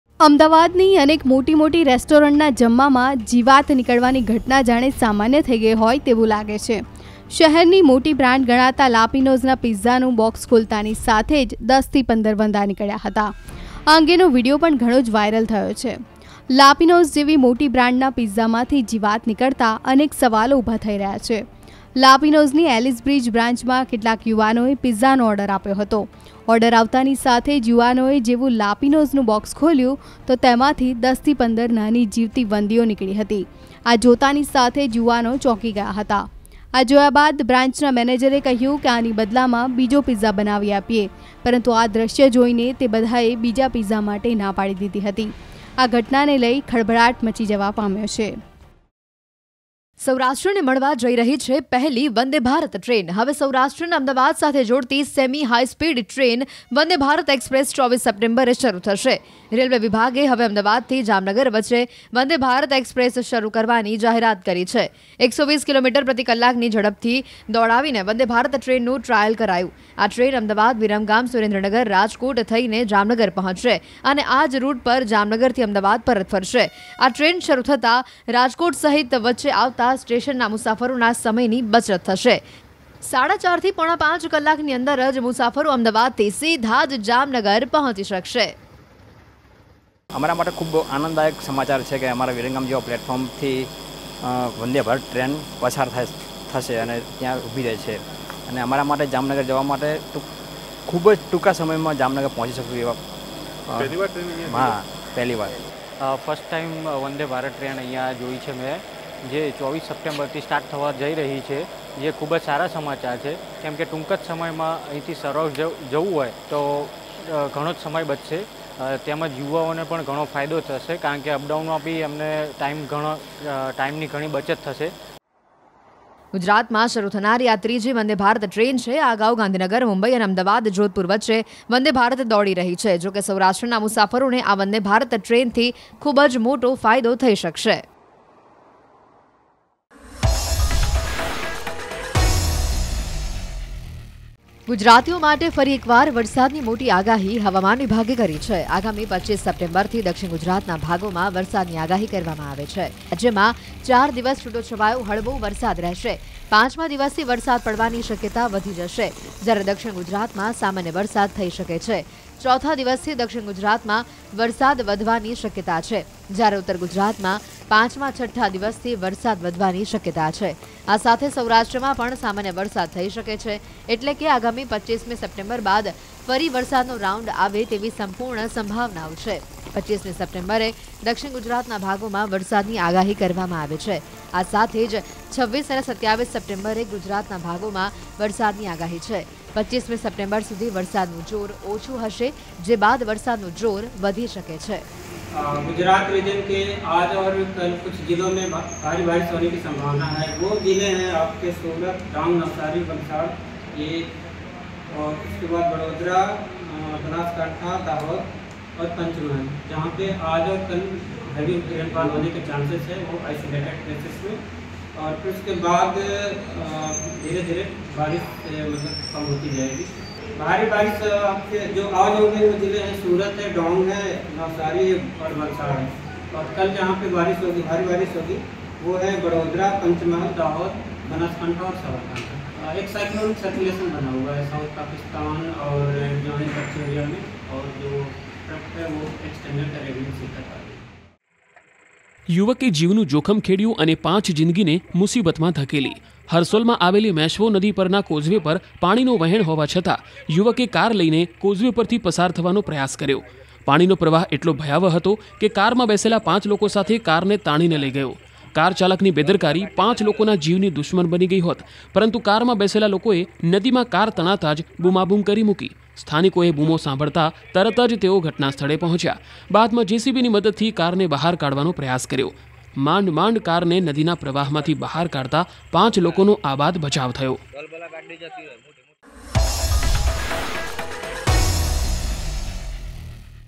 Top News Today: 4 મિનિટ 4 ખબર | 4 મિનિટમાં સૌથી મોટી 4 મહત્વની ખબરો | Gujarati News | News18 | N18V
અમદાવાદીઓ પિઝા ખાતાં ચેતજો...
સૌરાષ્ટ્રમાં દોડશે વંદે ભારત ટ્રેન
હવામાન વિભાગે કરી વરસાદની મોટી આગાહી
કાર નદીમાં તણાઈ ગઈ, 5 લોકો માંડ બચ્યા